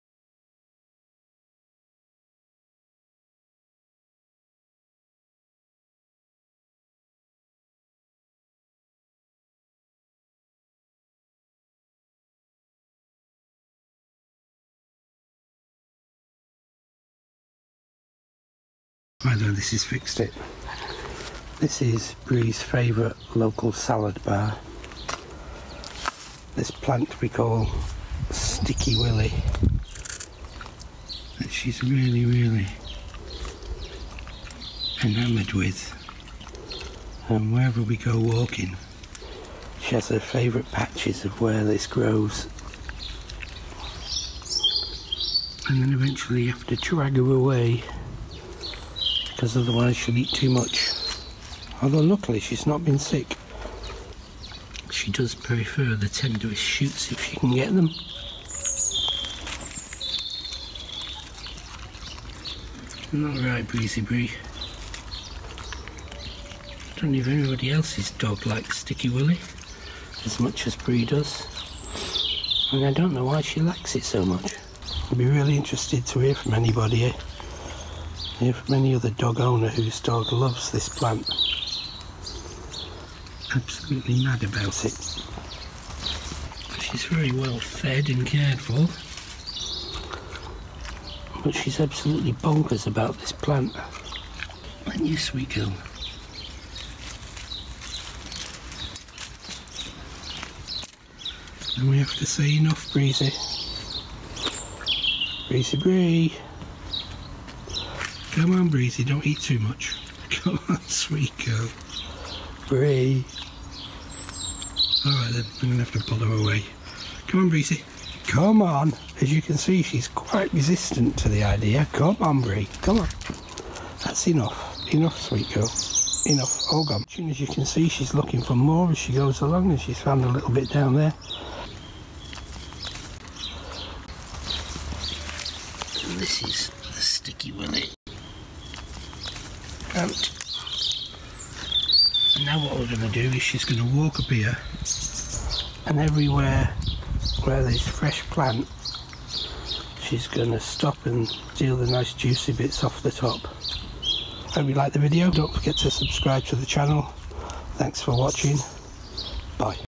Does Your Dog Love Goose Grass AKA Sticky Willy?
If this video helps you can buy @fixed1t a coffee Thx. My amazing rescue sight hound BREE is a Lurcher: A Saluki Greyhound Cross.  She is loyal, inquisitive, intelligent, caring (she prompts me when my blood sugar goes low) and great to walk but she has a dark secret

BREE is addicted to SALAD

and no matter how hard we try and what route we try to take to rid her off this habit she always brings us forcefully to one of her local Salad Snack Bars

The thing is she only likes the finest cleanest shoots, she won\t touch any that dogs have scented.  Whats odd is that Slippy our Tri-Paw Lurcher won't touch the stuff.  I even try eating some of it to see what it tastes like, and not only is it very bitter, akin to an astringent but the leaves have incredibly tiny hooks or bards on them that sticks it to you tongue and throat.  But BREE is not deterred and come rain or high water she's out on the hunt for her Salad.

My question to other dog owners, is your dog addicted to? :
    Sticky Willy
    Goose Grass
    Cleavers
    Clivers
    Galium Aparine

History also has is that it was used to pack sacks as a mattress.

Does Your Dog Have A 'Sticky Willy' Addiction?

Malcolm aka @fixed1t​